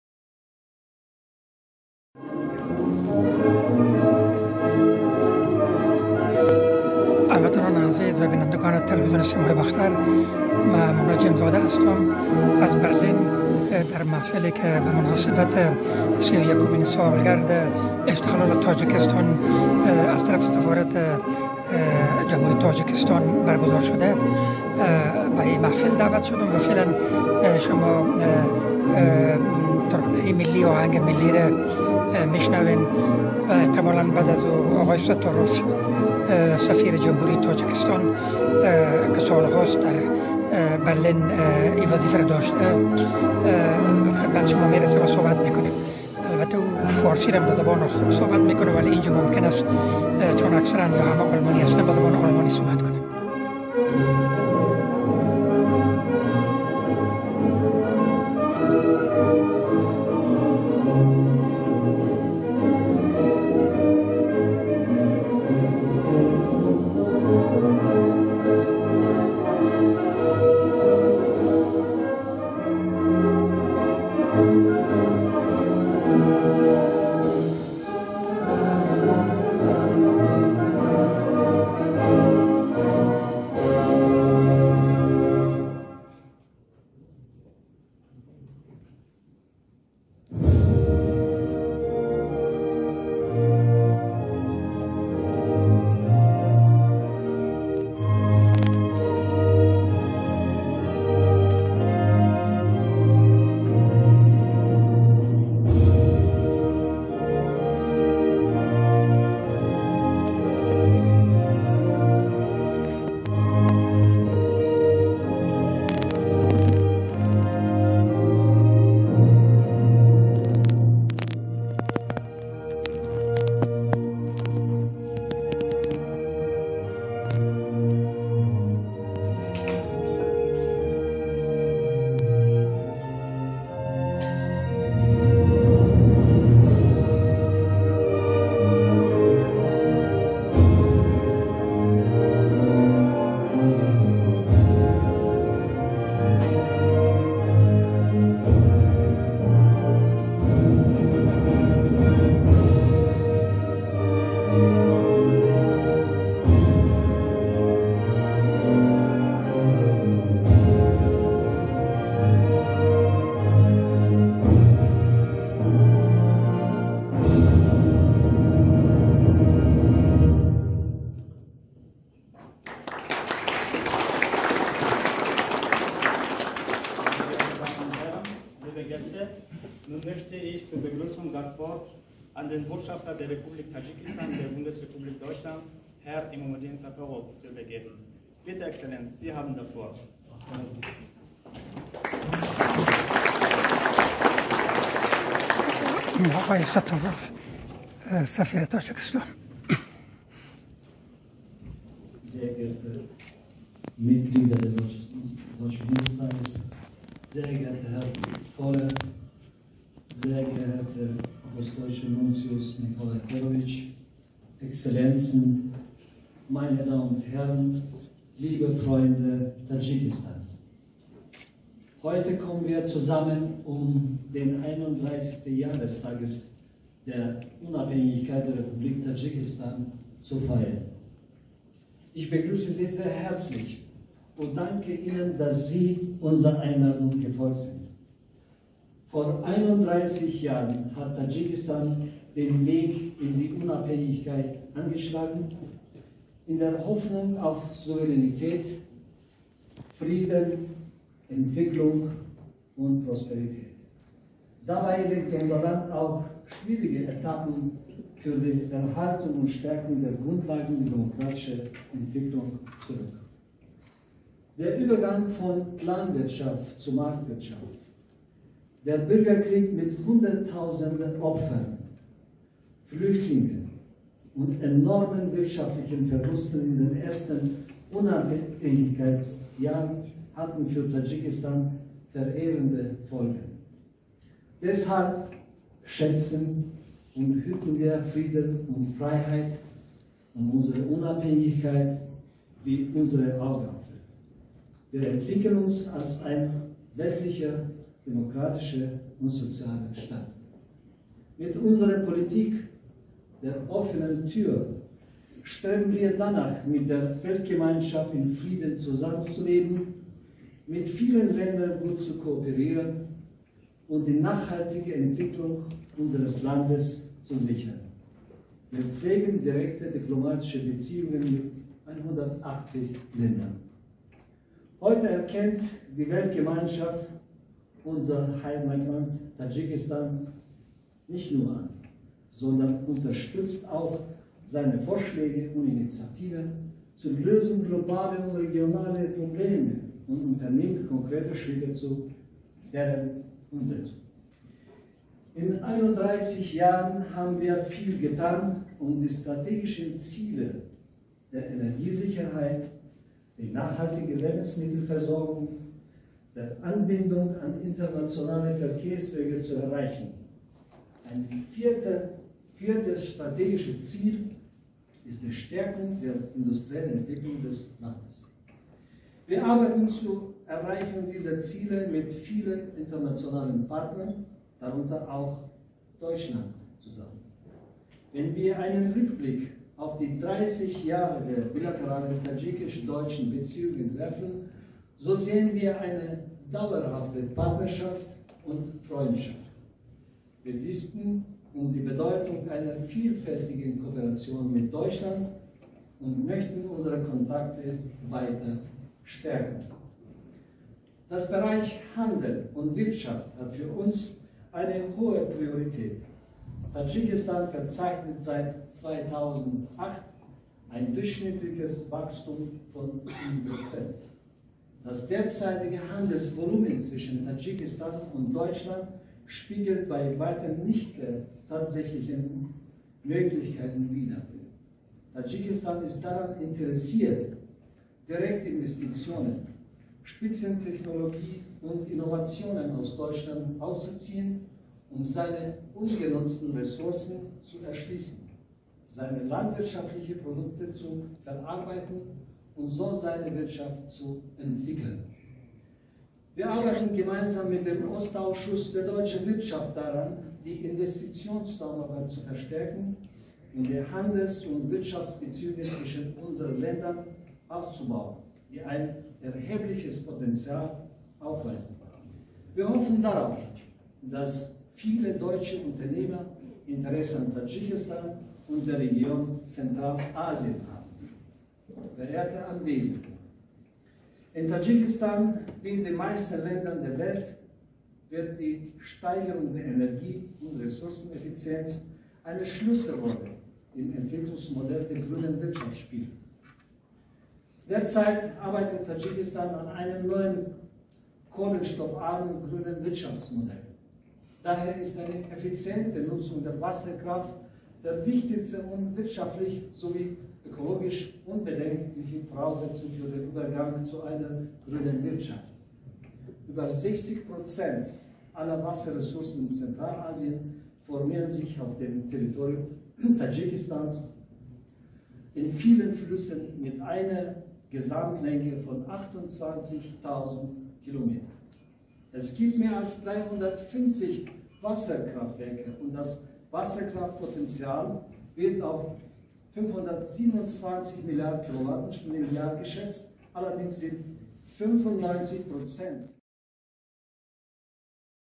همایش سی و یکمین سالروز استقلال تاجیکستان در برلین
به مناسبت سی و یکمین سالروز استقلال تاجیکستان در برلین  همایشی با کشورهای همسایه تاجیکستان و کشورهای آسیایی میانه در برلین بگ‌ذارشده بود که نخست اقای ستار اوف سفیر تاجیکستان به همین مناسیت سخنانی ایراد کرد .  در این همایش شماری از افغانستانی های مقیم المان نیز دعوت شده بودند  ولی سفیر افغانستان  در برلین در این همایش حضور نداشتد